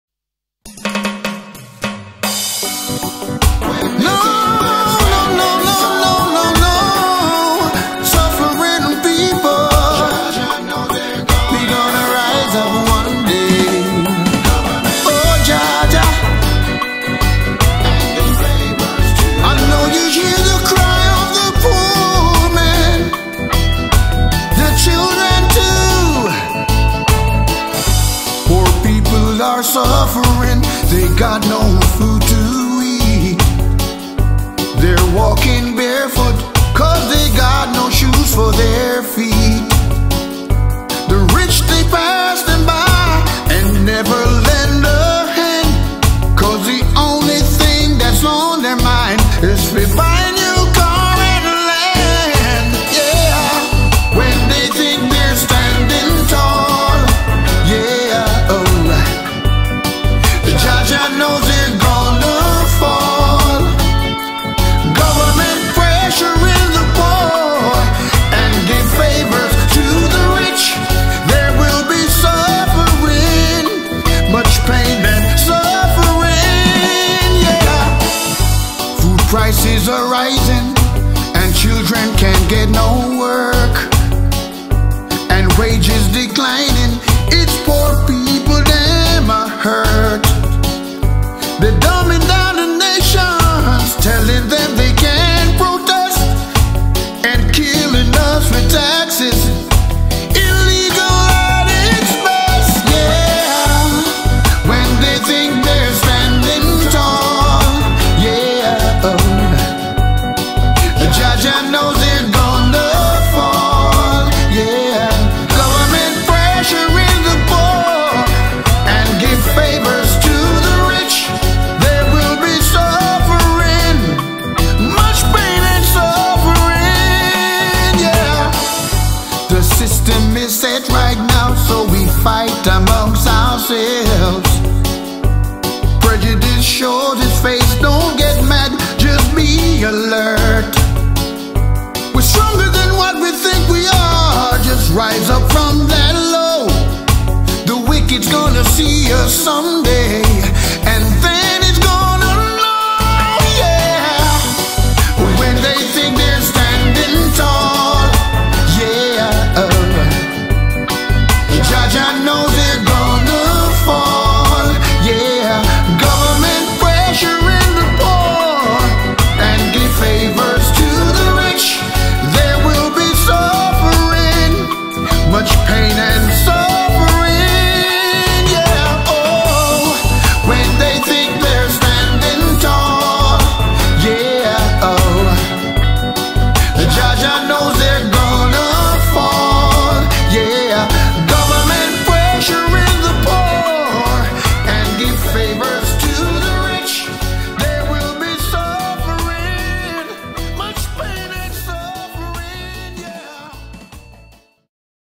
POOR PEOPLE
Governments around the world are creating poverty, instead of "Making Poverty History". They have developed the slave master approach to governing the people who "ELECTED THEM" WHY?